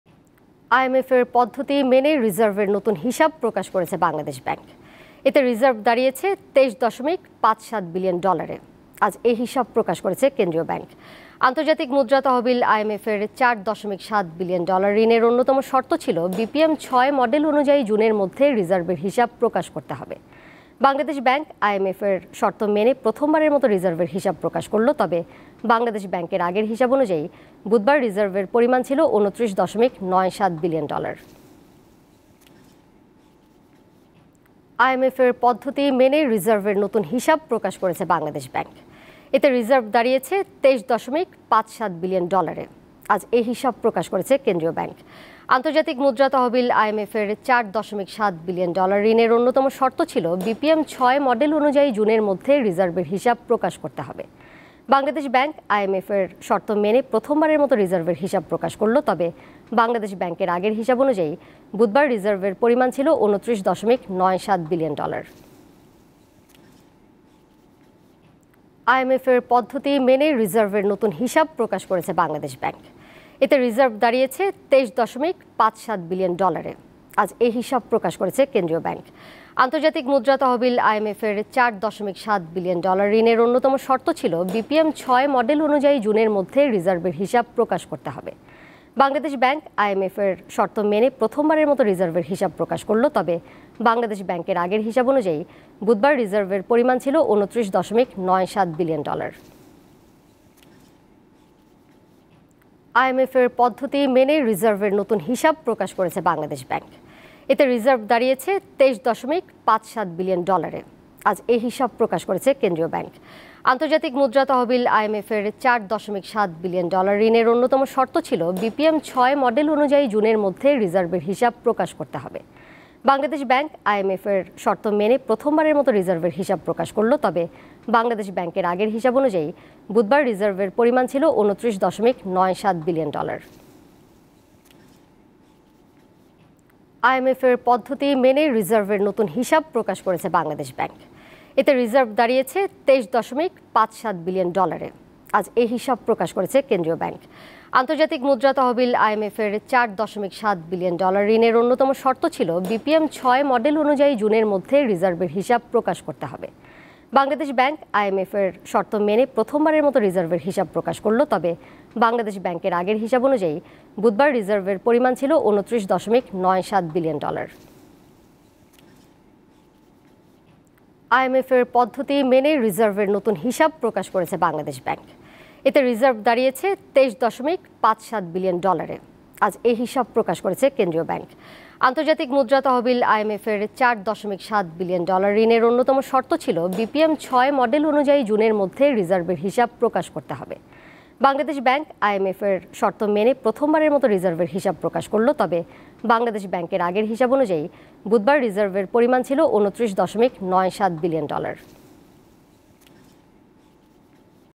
বাংলাদেশ ব্যাংকে নতুন রিজার্ভ ২৩ ৫৭ বিলিয়ন ডলার || Reserves || Deepto News
বাংলাদেশ ব্যাংকে নতুন রিজার্ভ ২৩ ৫৭ বিলিয়ন ডলার

Address:
Deepto TV
7/A/GA Tejgaon Industrial Area,